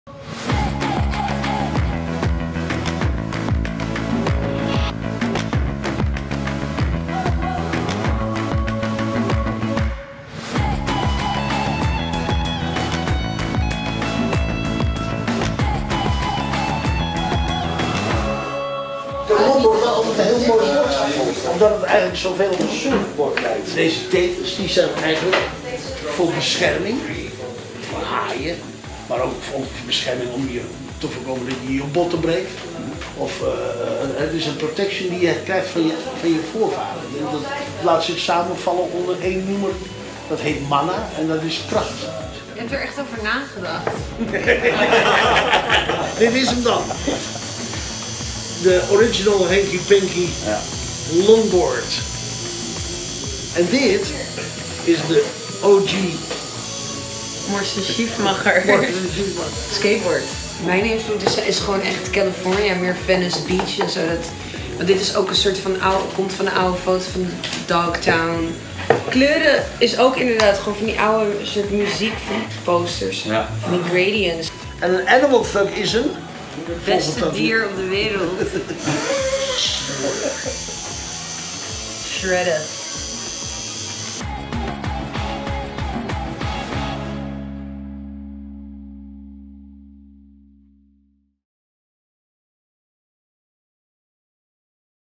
Henk Schiffmacher & daughter Morrison designed a limited edition longboard and skateboard
International tattoo artist Henk Schiffmacher and his daughter Morrison designed an unique and limited longboard and skateboard for the Dutch brand OneZeroZero Boards.

LIMITED EDITIONS & COLLECTORS ITEM:

- ONLY 100 boards will be produced.
- Real hand signature from Henk on your “Hanky Panky” Longboard or from Morrison on your "Animal Thug"
- Real hand signature from Henk and Morrison on certificate.
- Boards  and certificate will be numbered ( 1-100)
- Aluminium Limited Edition Badge mounted on this longboard